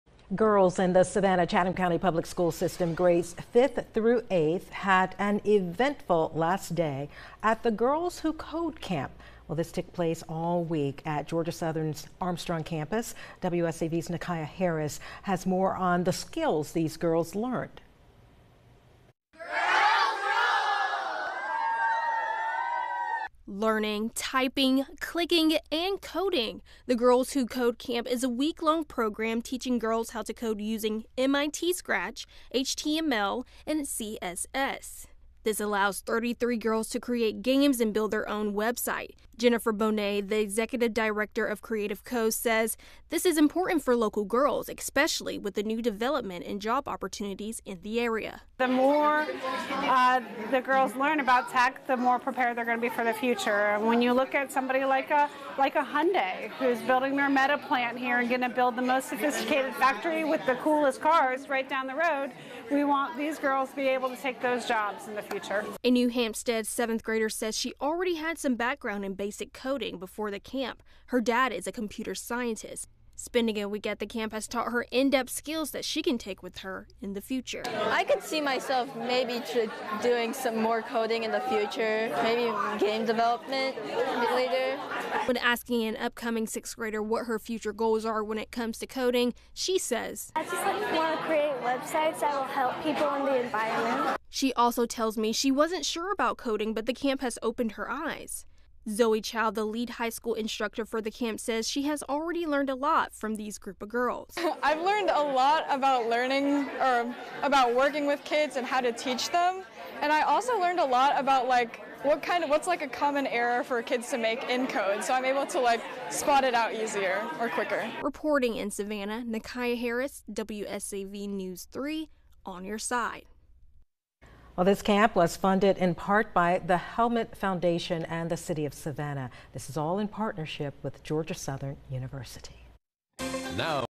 Girls Who Code camp wraps up Friday afternoon
Girls in the Savannah-Chatham County Public School System grades fifth through eighth had an eventful last day at the “Girls Who Code Camp” Friday afternoon.